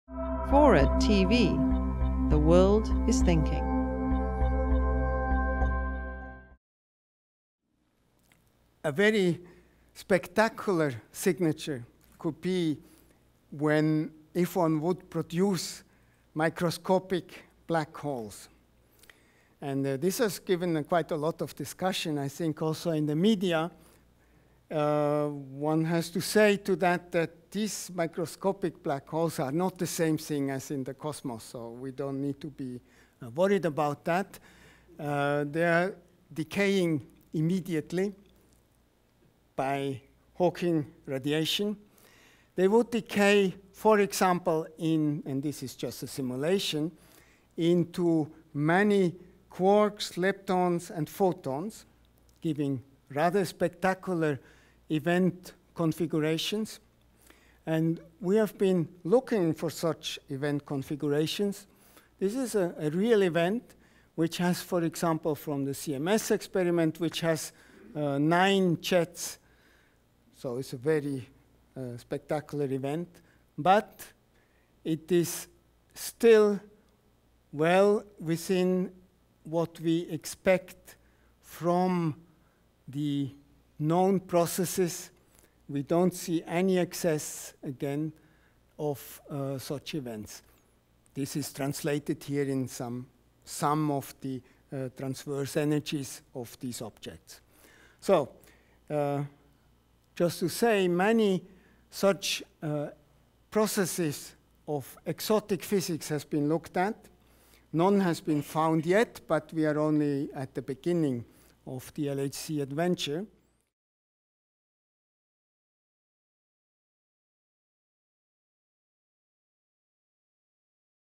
CERN's Quest to Create a Microscopic Black Hole
Following the discovery of the Higgs Boson particle, CERN scientist Peter Jenni discusses future projects the ATLAS group is working on at the Large Hadron Collider at CERN.